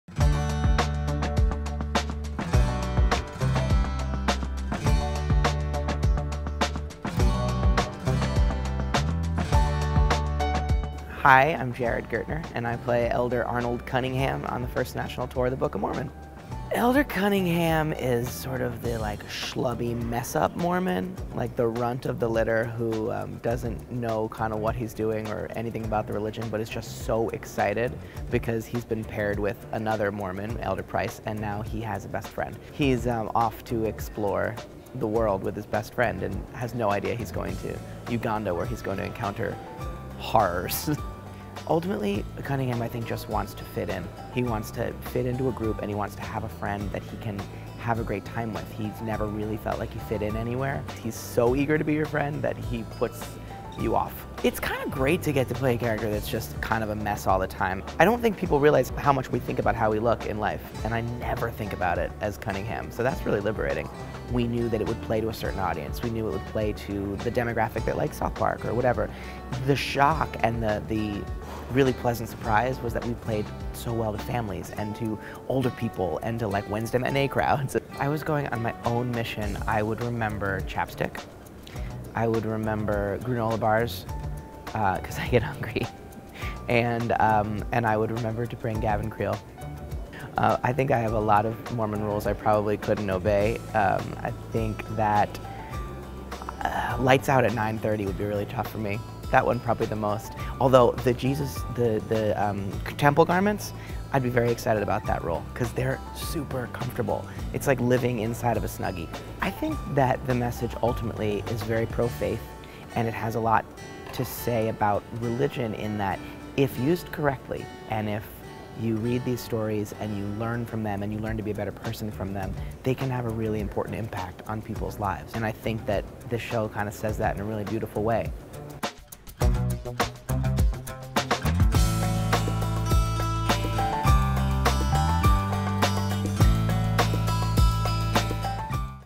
Jared Gertner Loves Being 'Schlubby' Elder Cunningham in "The Book of Mormon"
Jared Gertner stars as Gavin Creel's mission brother in the national tour of "The Book of Mormon." Listen to the actor talk about being "schlubby" on stage and the show's "pro-faith" message.